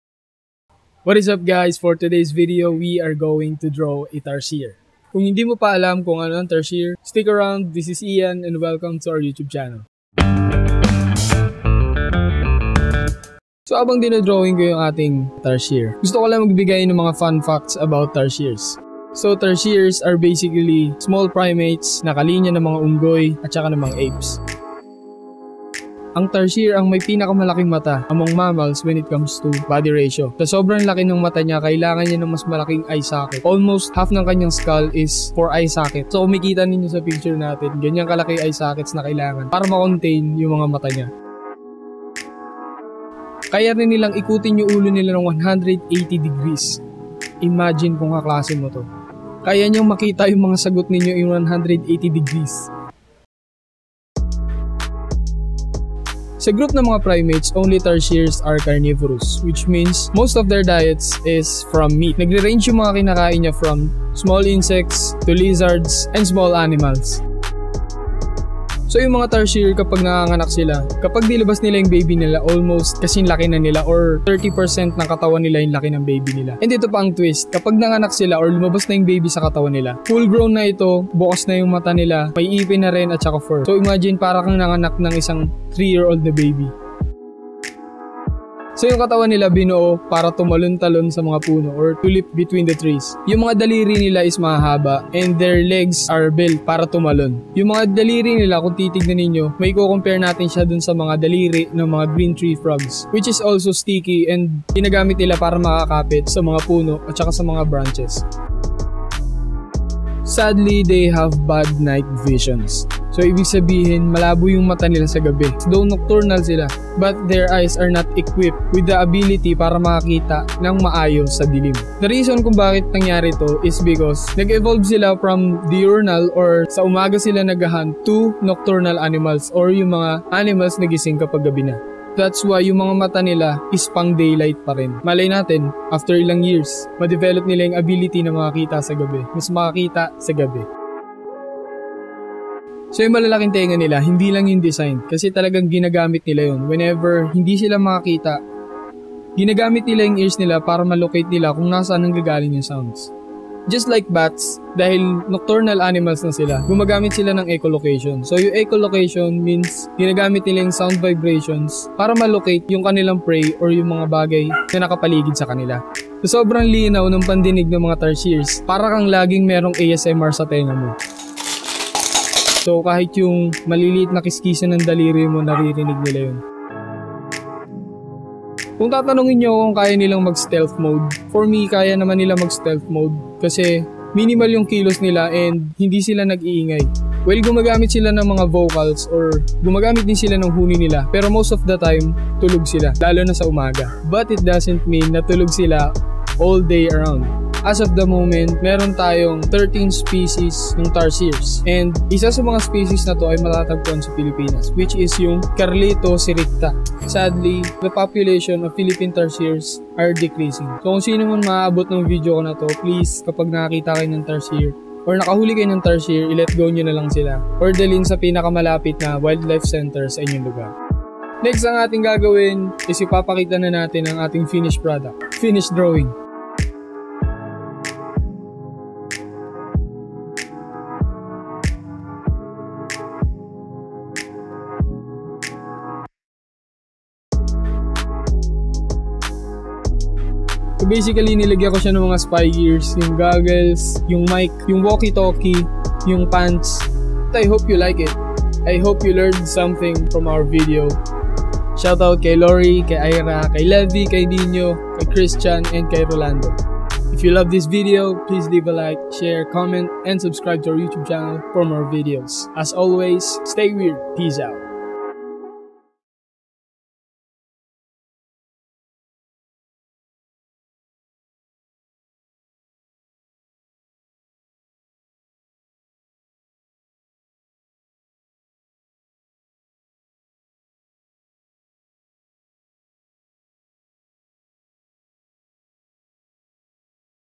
TURNING A TARSIER INTO A SPY AGENT AAAAAACCCKKK !!!
Our video for today will show things that may or may not know about Tarsiers. Join me as I turn this guy into a spy agent.

If you have comments and suggestions drop it on the comment section below. ;)

iANs ARTery